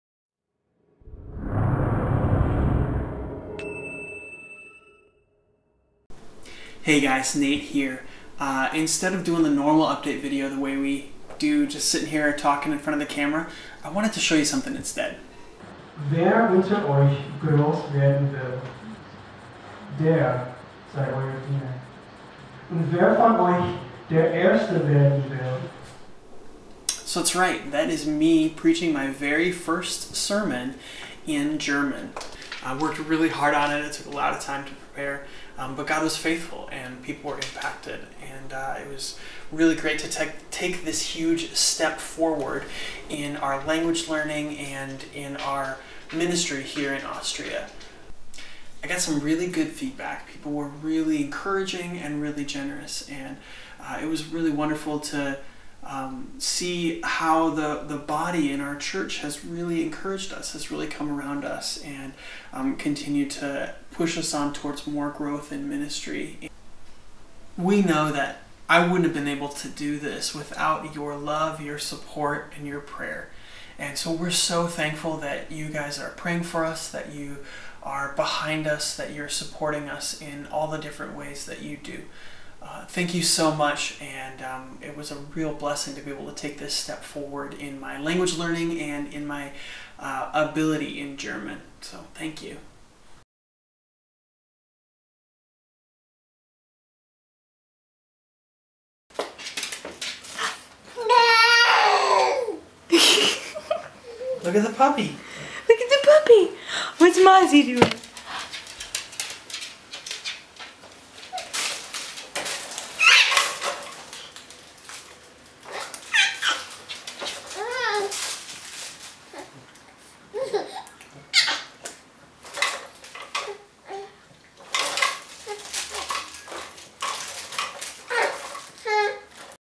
A Milestone in Language Learning
Hey guys, instead of doing our normal talking video update, I wanted to show you something instead. Here is a short clip of my first sermon in German preached at the Meidling church here in Vienna. It was a huge challenge, but God was faithful and it was really well received. Thanks so much for your prayers and support.